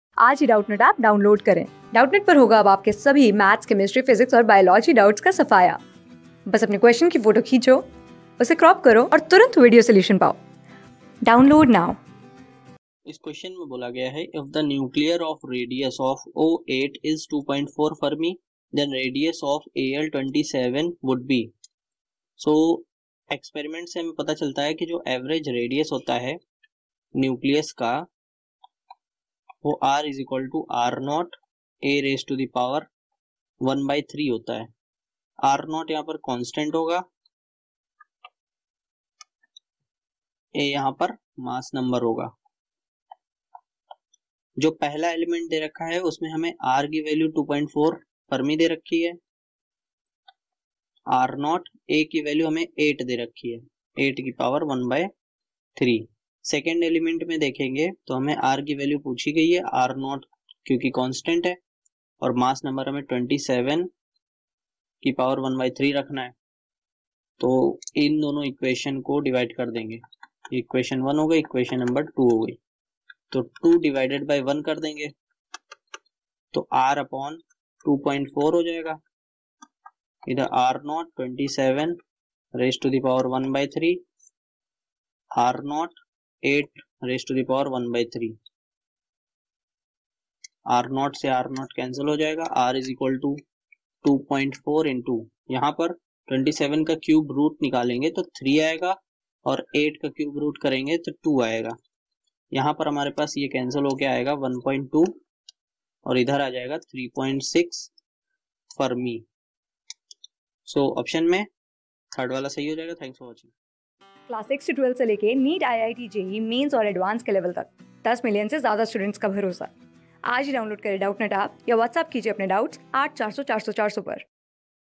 If the nuclear of radius of O^8 is 2.4 fermi, then radius of Al^27 would be | CLASS 12 | Mock Te...
If the nuclear of radius of O^8 is 2.4 fermi, then radius of Al^27 would be
Class: 12
Subject: PHYSICS
Chapter: Mock Test 39: PHYSICS
Board:IIT JEE

👉 Have Any Query? Ask Us.